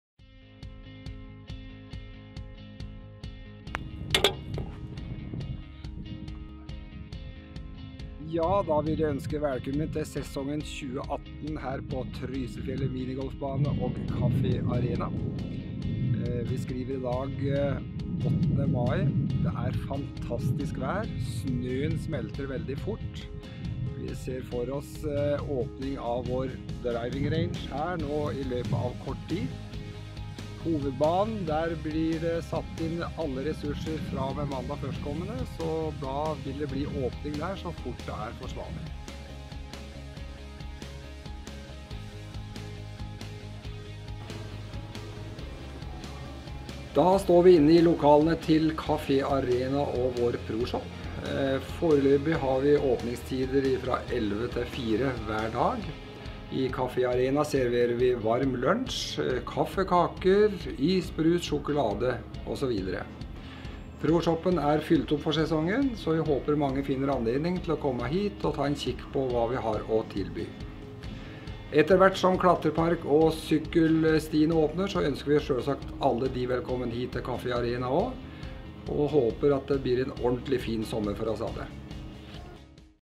Cafe Arena og minigolfbanen er åpen
Det våres i Trysil, og sommeraktivitetene begynner så smått og komme i gang. Nå er Cafe Arena og minigolfbanen åpnet. Juul Arne Norum, daglig leder for Trysilfjellet Golf gir også også status for vår 18-hulls golfbane.
Film: Jonas Sjögren